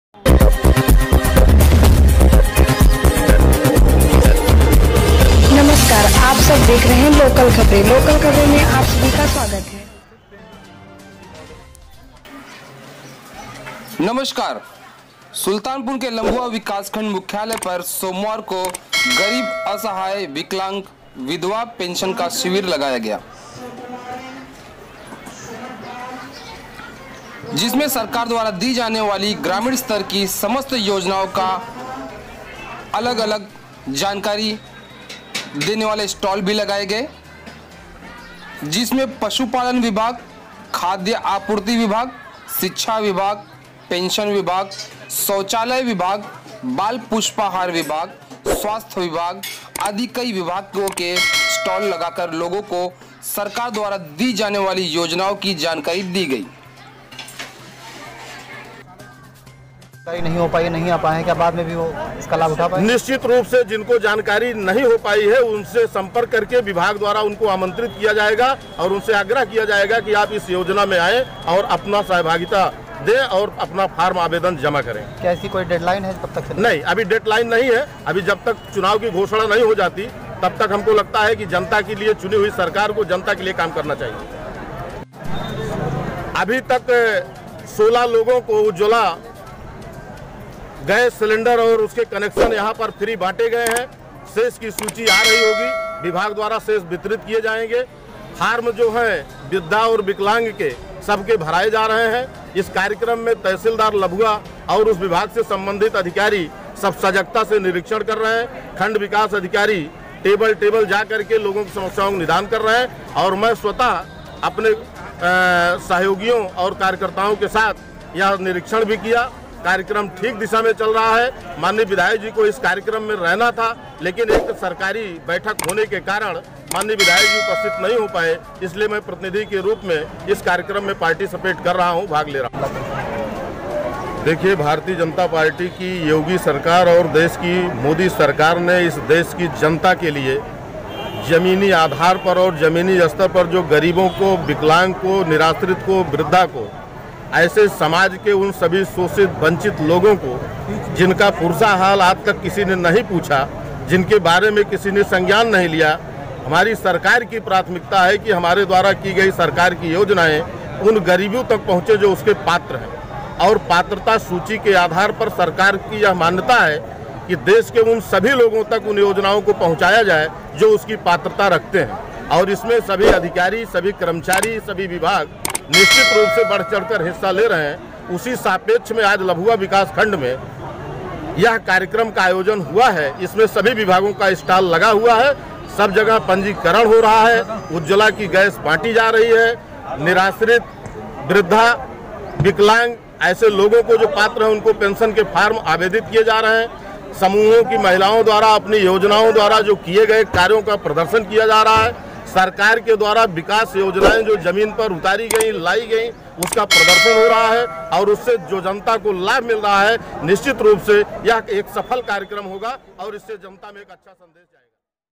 सुलतानपुर - लंभुआ विकासखंड मुख्यालय पर सरकार की विभिन्न योजनाओं की प्रदर्शनी लगायी गयी ...
Date - 28/1/2019

Report - वागीश कुमार

Place - सुल्तानपुर/लंभुआ

 सुल्तानपुर - लंभुआ
 विकासखंड मुख्यालय पर सोमवार को गरीब असहाय विकलांग विधवा पेंशन का शिविर लगाया गया जिसमें सरकार द्वारा दी जाने वाली ग्रामीण स्तर की समस्त योजनाओं का की जानकारी के लिए अलग-अलग स्टॉल लगाए गऐ जिसमें पशुपालन विभाग खाद्य आपूर्ति विभाग शिक्षा विभाग पेंशन विभाग शौचालय विभाग बाल पुष्टाहार विभाग स्वास्थ्य विभाग आदि कई विभाग के स्टॉल लगाकर लोगों को सरकार द्वारा दी जाने वाली योजनाओं की जानकारी दी गई शिक्षा विभाग के स्टॉल पर विकलांग बच्चों के लिए  विशेष जानकारी दी गई और सरकार द्वारा मुफ्त में दी जाने वाली सुविधाओं  की जानकारी भी दी गयी ।कार्यक्रम  की शुरुआत लंभुआ क्षेत्र के विधायक देवमणि दुबे के प्रतिनिधि शिवाकांत ने की उन्होंने बताया कि सरकार द्वारा जो भी सेवाएं या सुविधाएं गांव के पिछड़े इलाकों तक पहुंचने चाहिए उन योजनाओं की जानकारी हम लोगों में जागरूकता उत्पन्न करने के लिए स्टार लगा कर दे रहे हैं हमारा प्रयास है सरकार द्वारा लोगों को मिलने वाली सुविधाओं का पूरा लाभ मिल सके। कार्यक्रम के दौरान विकलांग वृद्धा व विधवा पेंशन फार्म के योग्य गरीब तबके के लोगों  का फॉर्म मुख्यालय परिसर में संबंधित अधिकारियों द्वारा जमा कराया गया और उन्हें आश्वस्त किया गया जल्द ही  उनकी पेंशन उन्हें मिलने लगेगी। कार्यक्रम के दौरान ब्लाक परिसर लगे स्टॉल का विधायक प्रतिनिधि शिवाकांत मिश्रा ब्लाक प्रमुख प्रतिनिधि जगदीश चौरसिया तहसील जितेंद्र गौतम नायब तहसीलदार शिव नरेश सिंह खण्ड विकास अधिकारी अमरनाथ शुक्ला पूर्ति निरीक्षक नन्हे सिंह एडीओ पंचायत दाऊद खान द्वारा निरिक्षण किया गया।